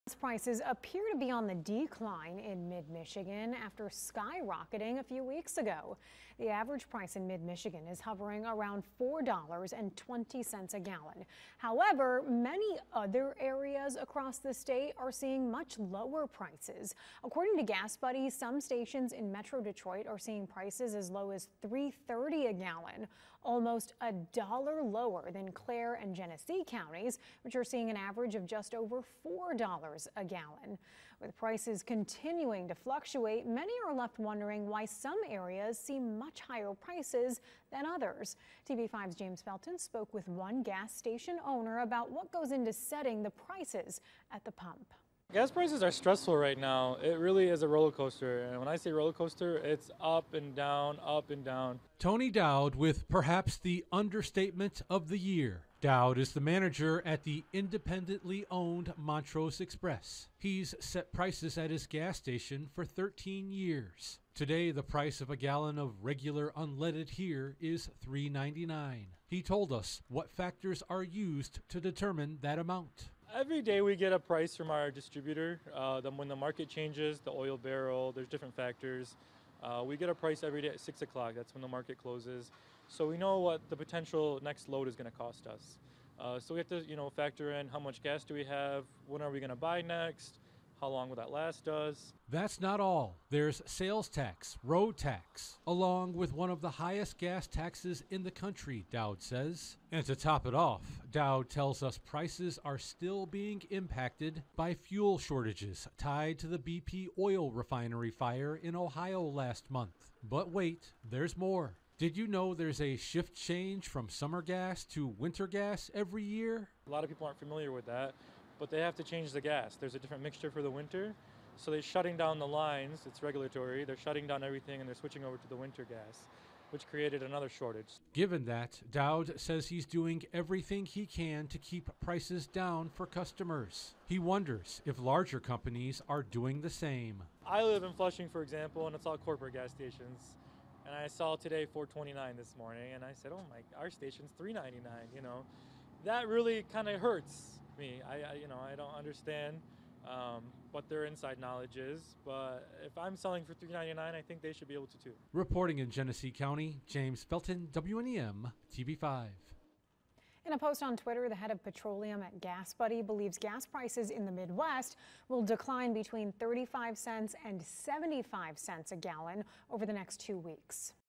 Gas station manager explains how he sets the price you pay at the pump
Gas prices appear to be on the decline in Mid-Michigan after skyrocketing a few weeks ago.